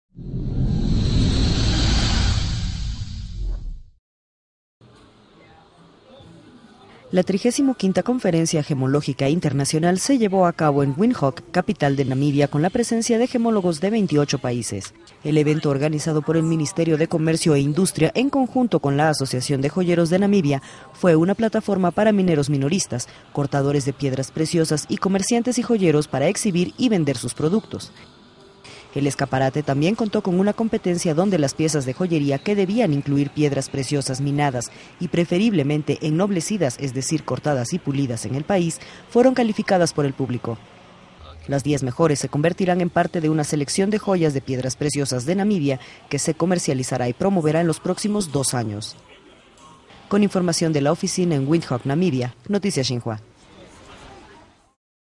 Celebran 35a Conferencia Gemológica Internacional
La 35ª Conferencia Gemológica Internacional se llevó a cabo en Windhoek, capital de Namibia, con la presencia de gemólogos de 28 países.